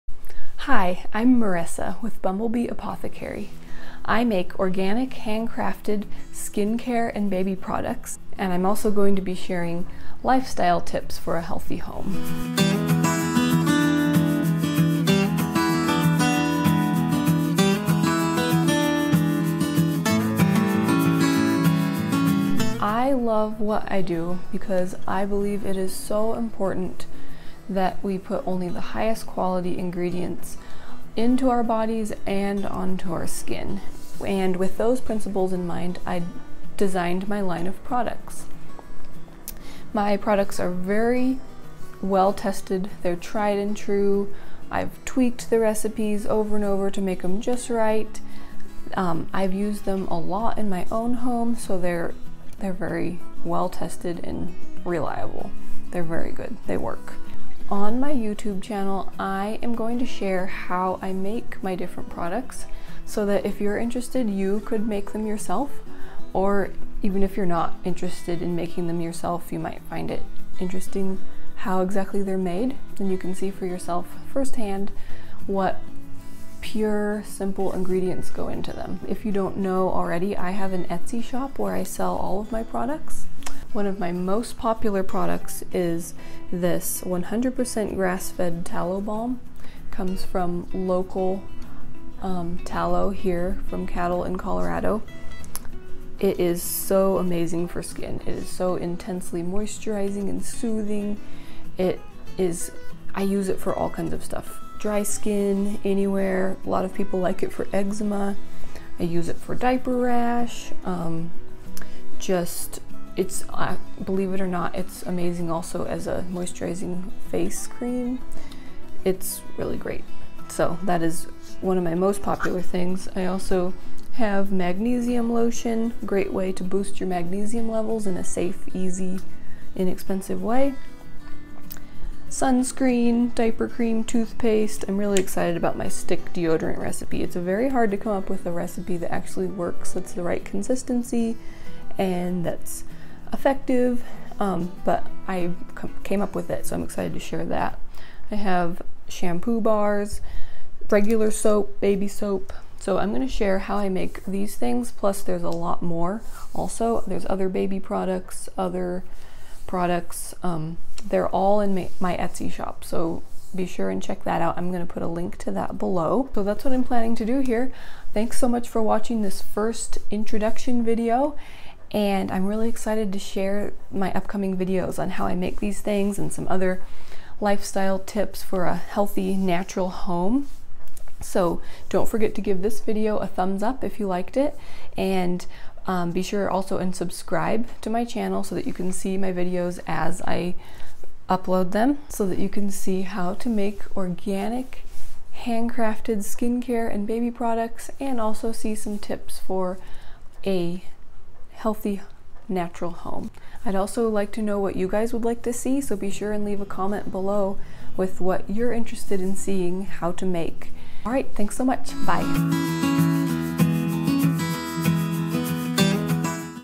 Meet Marisa | Owner of Bumblebee Apothecary | Organic Skincare DIY
Hello! I'm so excited to meet everyone! Join me as I show you how to create organic, handcrafted skincare and baby products.

🐝 Grab my FREE DIY Organic Skincare Recipes eBook 1

🐝 My current video equipment:
Canon EOS 80D

-Marisa 🐝